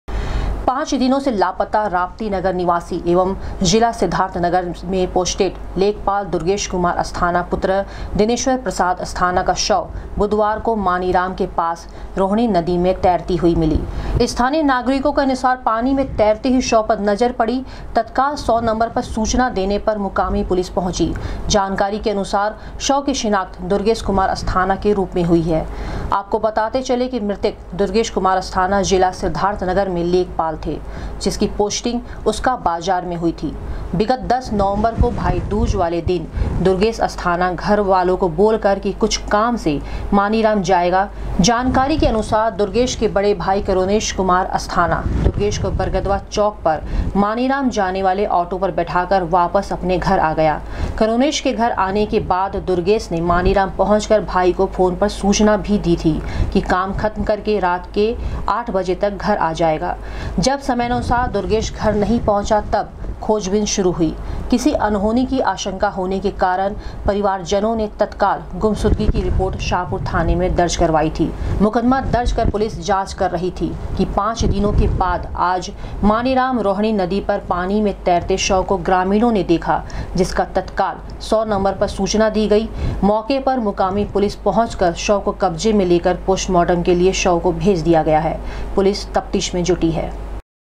पांच दिनों से लापता लेखपाल का शव संदिग्ध परिस्तिथियों में थाना चिलुआताल क्षेत्र में रोहणी नदी
पांच दिनों से लापता लेखपाल का शव संदिग्ध परिस्तिथियों में थाना चिलुआताल क्षेत्र में रोहणी नदी से हुआ बरामद , पुलिस ने शव को कब्जे में लेकर पोस्टमार्टम के लिए भेजा, पुलिस जुटी तफ्तीस में